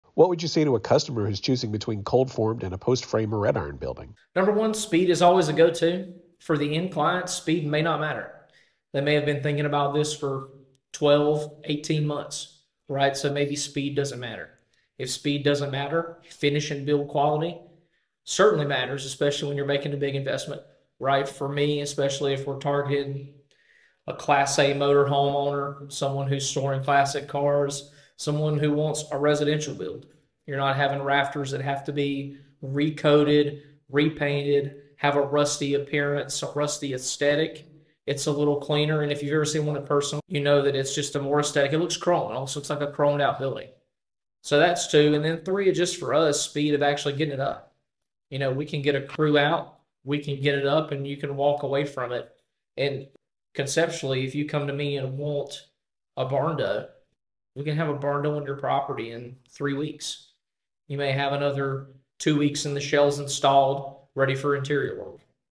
What are the advantages of COLD FORMED over post frame and red iron?
Hunter Jeffcoat from Quicken Steel talks about the advantages of buying and building with COLD FORMED steel.